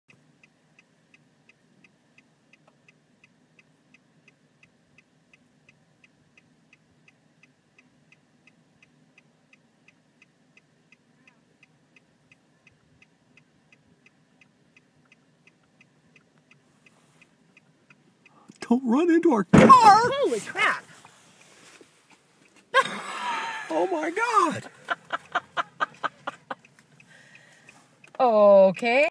You won’t believe what these Yellowstone buffalo do!!!
Bison charge parked car from 200 yards in Yellowstone National Park’s Lamar Valley (The Serengeti of North America). Video by Tom Carter from vehicle owned by Suzie Hollingsworth. According to AAA Insurance, damage to the car totaled $2,787.82. That's more than a dollar per pound of the 2,000 pound bison!